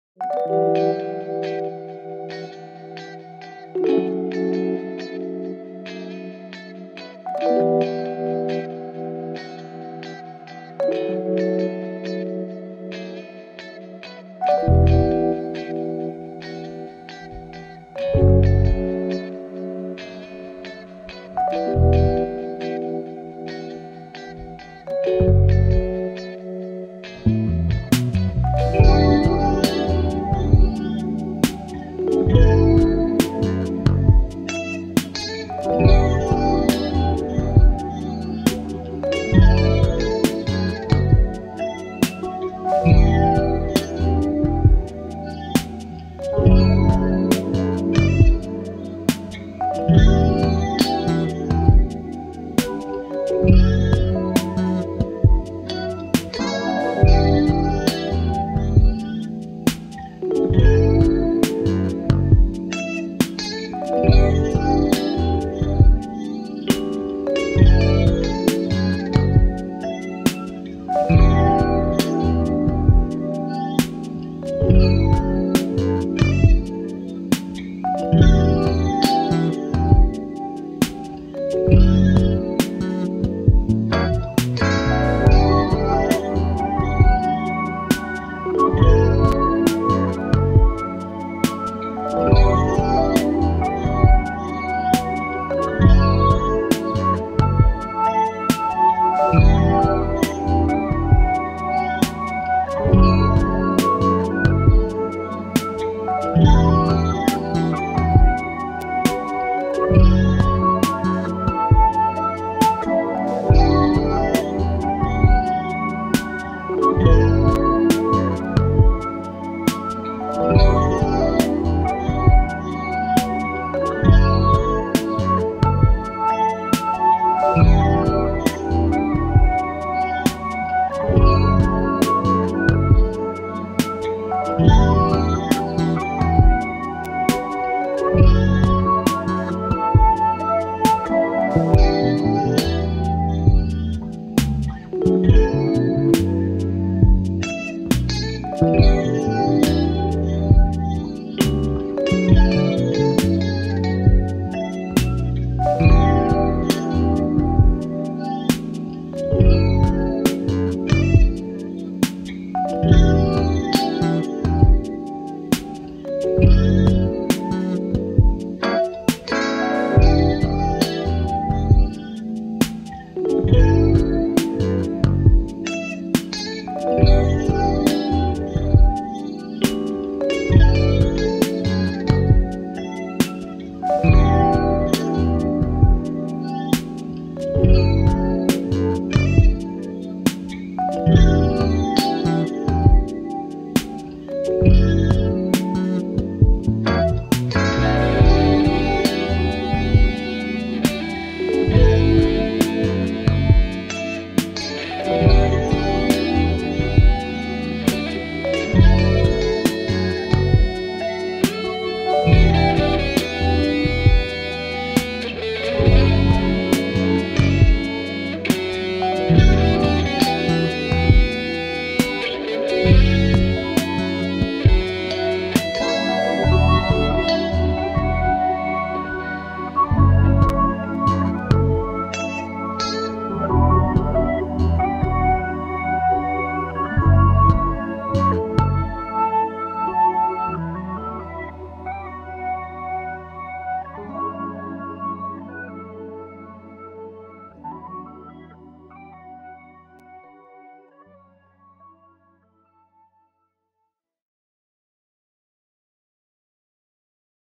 Change Lips color in Retouch Pro Panel ( Photoshop )
Best Way To Learn How To Retouch - Step by Step in Photoshop Tutorial.